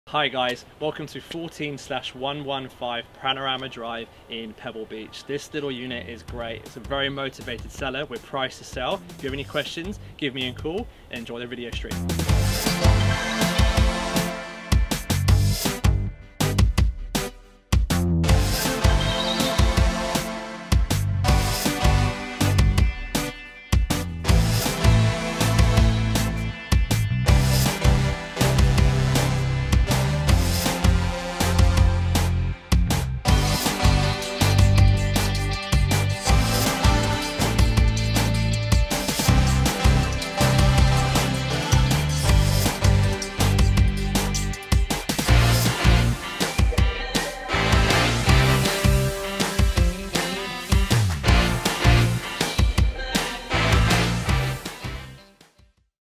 Quiet Beach & Golf Course Lifestyle Awaits
14/115 Panorama Drive, Preston Beach

Its all about the memories. This family beach retreat is surrounded by memories for the family. Whether its sitting back and enjoying a break or playing a round of golf, that is right on your door step or a day of fishing or taking the 4WD to the beach? The location is something that wont disappoint and will give you many family memories for years to come!

This 2 bed, 1 bathroom beach house has an open plan kitchen and dining area with vaulted ceilings and a private garden area to watch the kids run a round and burn some energy. Preston Beach has accommodation and facilities with a couple of roadhouses located on Old Coast Road just 4 km from the town. Within the Preston Beach town site a convenience store and service station can also be found. The amazing beach has showers, BBQ facilities and a children's play area.

Don't miss this opportunity contact Marcus Blake on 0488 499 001 for more information and to arrange a viewing today.